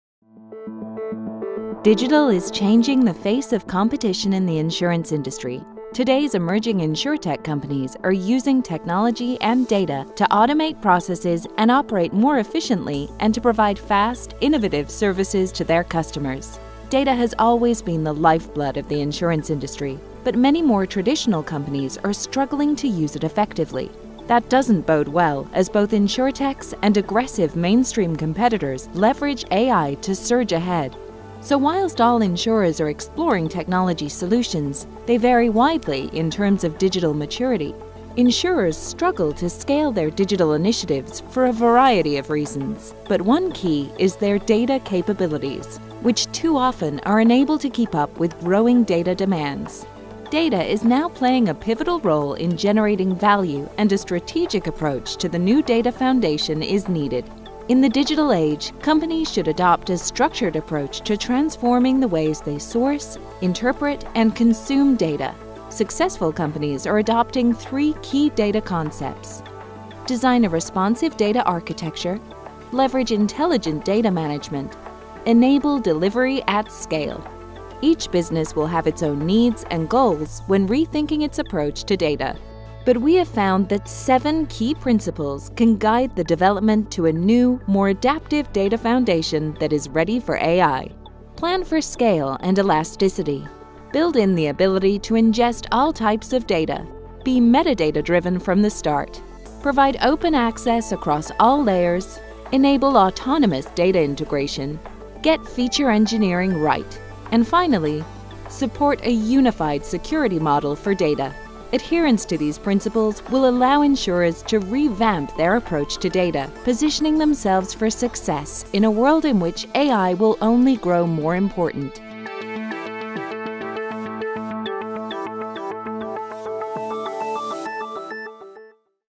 The Importance of Data for Insurers | Cognizant
Data plays a pivotal role in generating value. In the digital age, insurers should adopt a structured approach to transforming how they source, interpret and consume data.